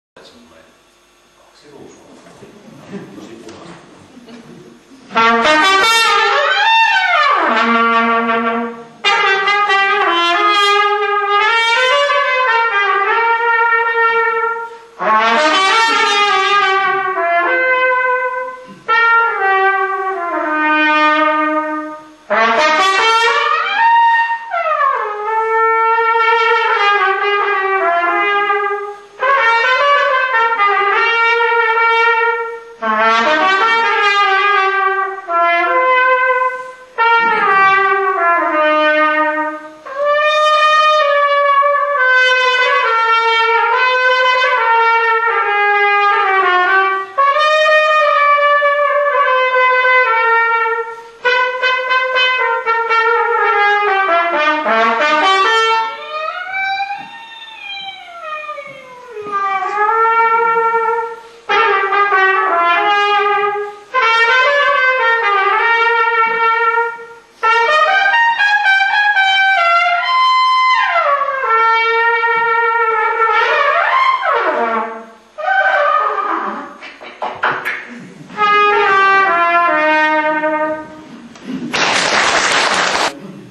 Juha Lauttamus, trumpet
Esiintyminen Ullan (vaimoni Ulla Lauttamus, ja Arin (Saastamoinen) vaali-iltamissa Jurvansalossa eduskuntavaalien aikaan 2007